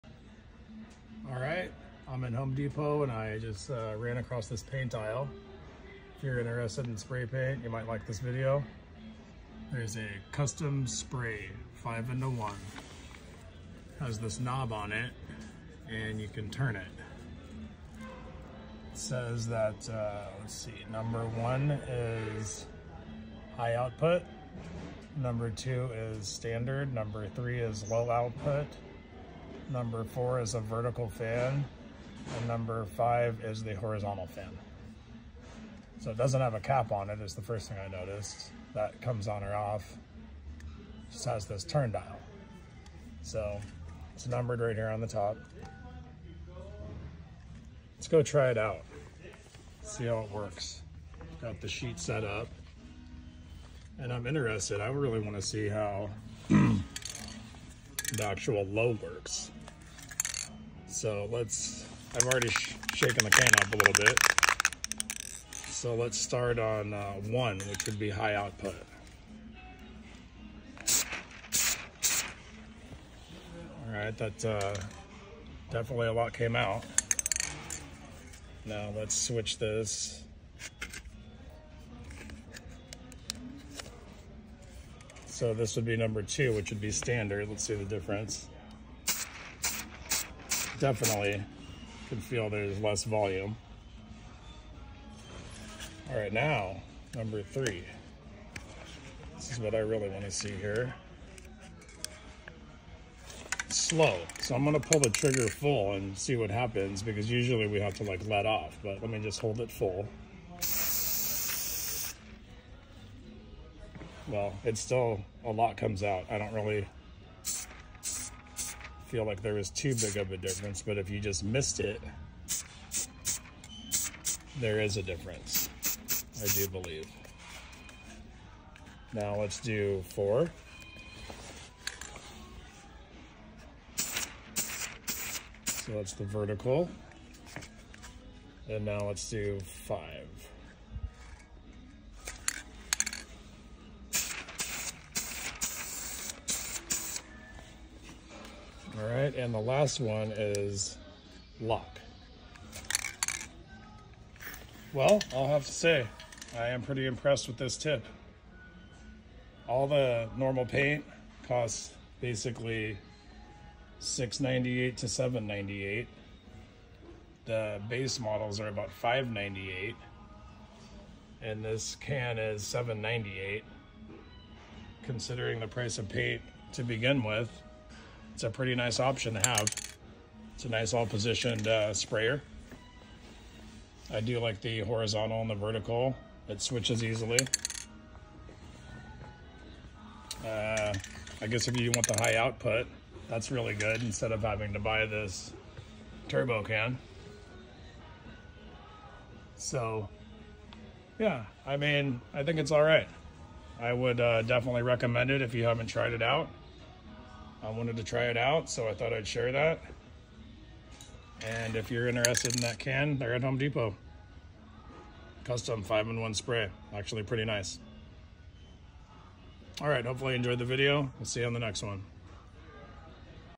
I tried the new 5 in 1 Spray Paint and this is how it worked! 😀
This is a cool new product! I spray a lot of paint . I must say this is pretty cool😀